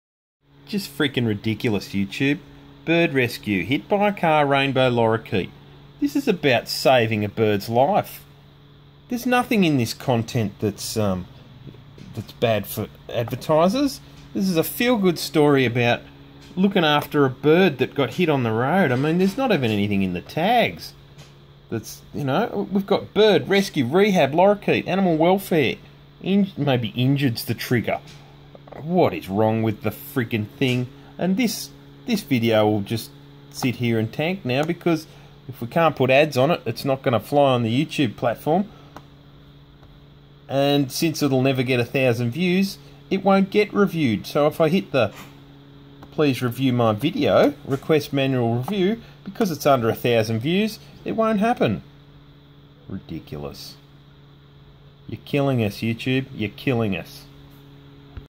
Youtube stupid bot. Not ad friendly...wildlife rescue? REALLY????
A feel good story about an injured bird being rehabilitated is not ad friendly? Come on youtube your destroying peoples ability to even have a post about even the most menial of things. I will re-edit the video so it is not the same length and re-upload. I will remove any negative words from the tags and see if it flies then. I am clicking monetize and I fully expect the bot will flag this video because of the tags "not ad friendly" and "bot".